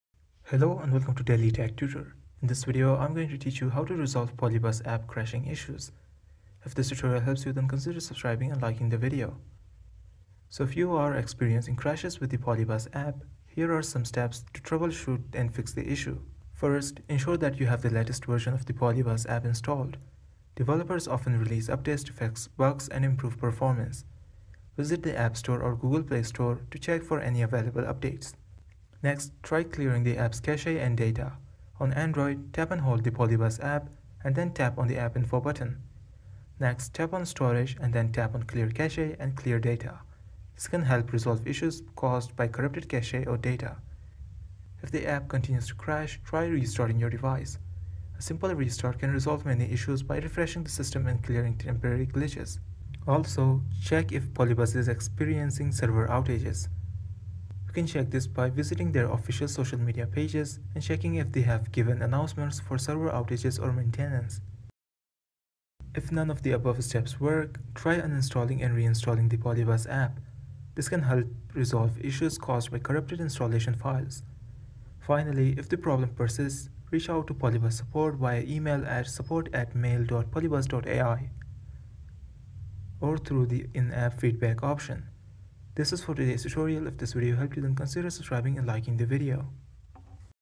How to Resolve PolyBuzz App Crashing Issues
Is your PolyBuzz app crashing or freezing when you try to create or interact with your AI characters? Don’t worry—you’re not alone, and this video is here to help! In this quick and easy guide, we’ll walk you through the most effective ways to resolve PolyBuzz app crashing issues so you can get back to building and enjoying your virtual experiences without interruption.

From minor glitches to more stubborn bugs, crashes can be frustrating—but they’re often easy to fix with just a few adjustments. Whether you’re on Android or iOS, this video covers the latest solutions that actually work in 2025.

Here’s what we’ll cover step by step:

Checking for app updates and installing the latest version

Clearing the app cache and stored data to boost performance

Reinstalling the app properly without losing your data

Ensuring your device software is fully updated

Managing background apps and system memory for smoother operation

Contacting PolyBuzz support if the problem persists

This guide is designed for both new and experienced users who want a fast fix without the tech jargon. You’ll be back to creating custom characters, assigning voices, and exploring the AI universe in no time!

- Team Daily Tech Tutor